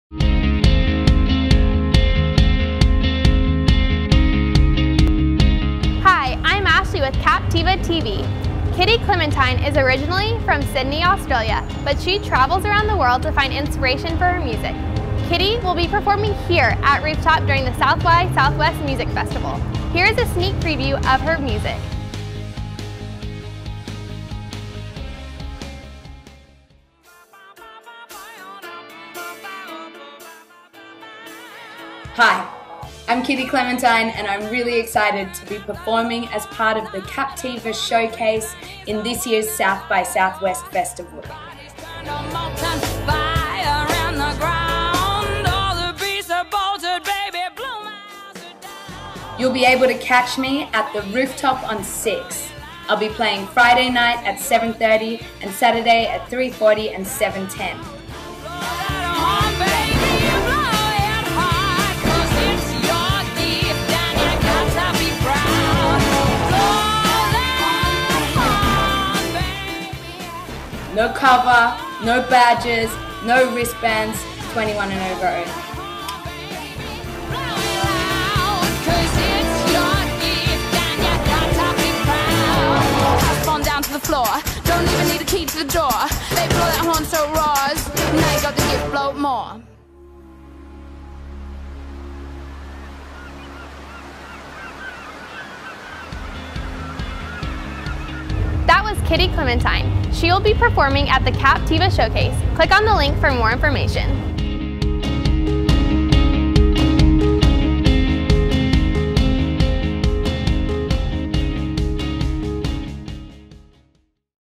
Captiva Showcase Artist Kitty Clementine - CaptivaTV
Australian Artist Kitty Clementine will be performing at the Captiva Showcase at Rooftop on 6th (403 E 6th) on Friday, March 15th & Sat., March 16th.

"I am Kitty Clementine, I make reality magical and I run hot on the heels of revelry. I am sharp, I am fierce, and I have stories you won't believe. So fluff up your feathers and run the road, be brave and wild with me"